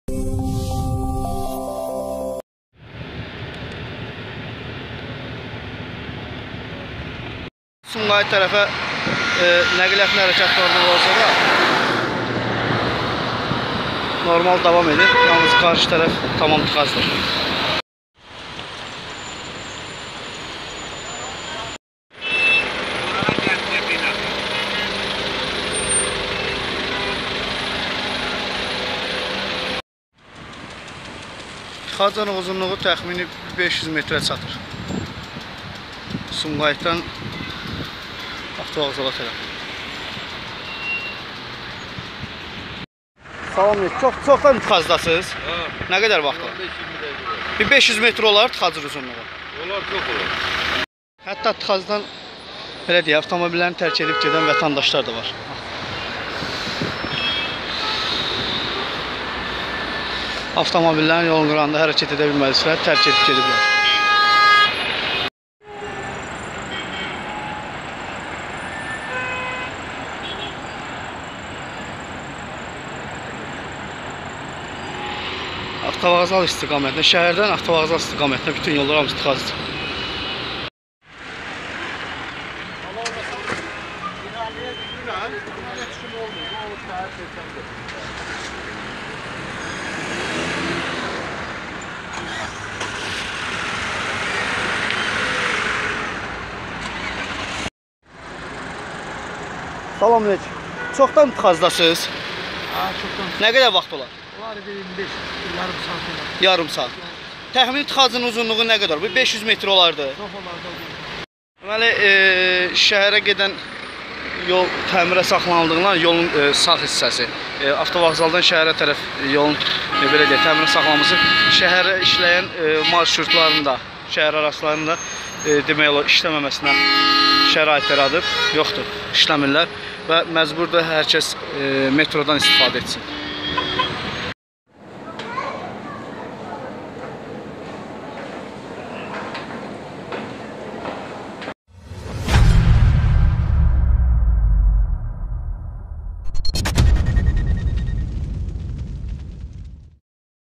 Bakı-Sumqayıt yolunda dəhşətli tıxac-Sürücülər çətin anlar yaşayıb
Tıxac səbəbindən vətəndaşlar Bakı-Sumqayıt yolunda çətin anlar yaşayıb.